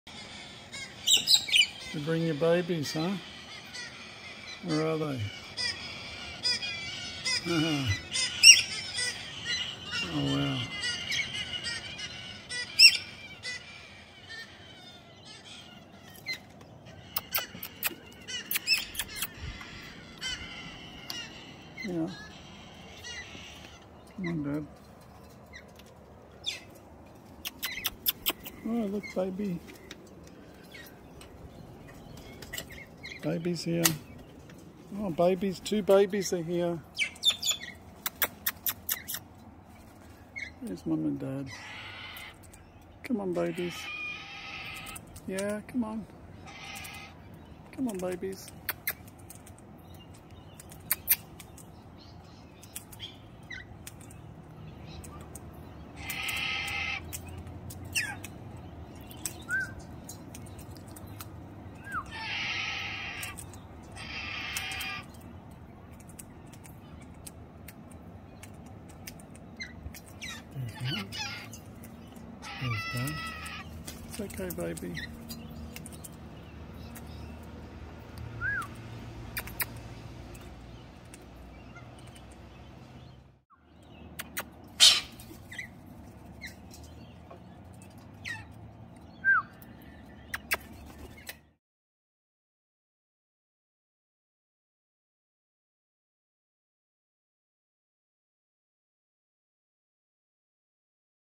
Ma and Pa Pink Galah 💕 SHOW OFF THEIR CUTE KIDS!!!👶👶💗💗🕊️🕊️🤗🥰😍😍(WARNING⚠️ VERY CUTE! 💗🤣💕)
Everyone loves Pink Galahs but there's nothing cuter than a BABY Pink Galah! What if there was 2 baby Pink Galahs? Well you're in luck because Ma and Pa Galah brought their 2 babies to show them off to me in this cute little video! 😍🥰🥰🤗

Please also check out some other amazing videos below 👇🏼